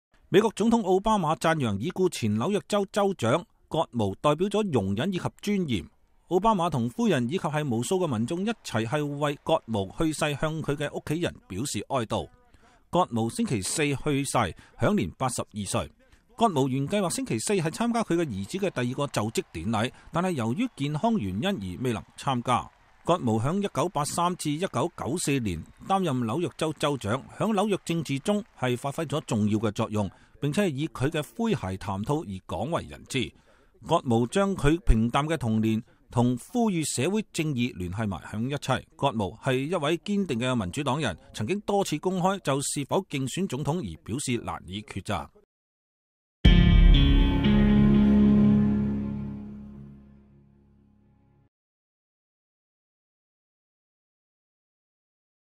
奧巴馬總統悼念紐約前州長葛謨
美國總統奧巴馬讚揚已故前紐約州長葛謨代表了容忍和尊嚴。 奧巴馬和夫人以及無數民眾一起為葛謨去世向他的家人表示哀悼。 葛謨星期四去世，享年 82 歲。葛謨原計劃星期四參加他兒子的第二個就職典禮，但由於健康原因而未能參加。